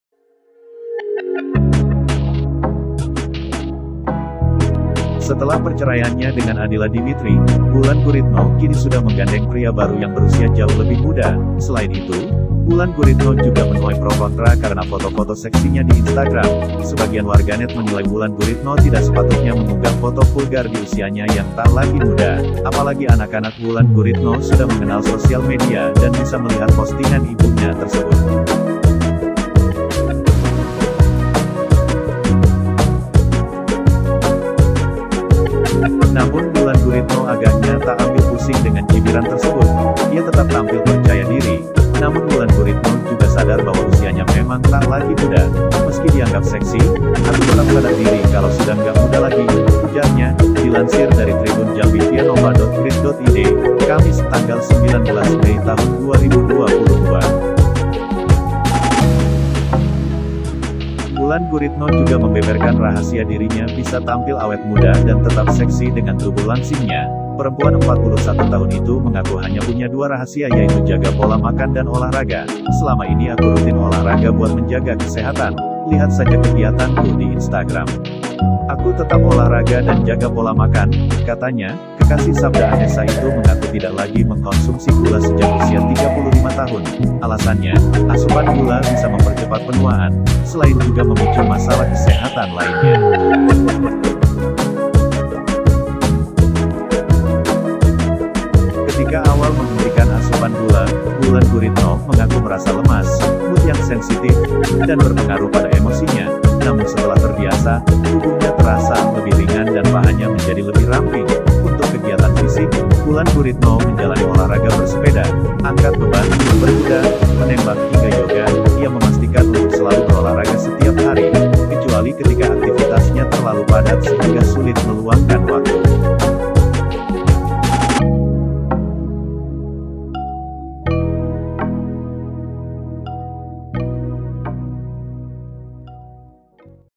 Wulan Guritno Tak Konsumsi ini Sejak Usia 35 Rahasia Awet Muda | Kini Gandeng Pria Berondong
Wulan Guritno Rahasia Awet Muda Tak Konsumsi ini Sejak Usia 35, Kini Gandeng Pria Berondong
Setelah perceraiannya dengan Adilla Dimitri, Wulan Guritno kini sudah menggandeng pria baru yang berusia jauh lebih muda.

Selain itu, Wulan Guritno juga menuai pro kontra karena foto-foto seksinya di Instagram.

Sebagian warganet menilai Wulan Guritno tidak sepatutnya mengunggah foto vulgar di usianya yang tak lagi muda.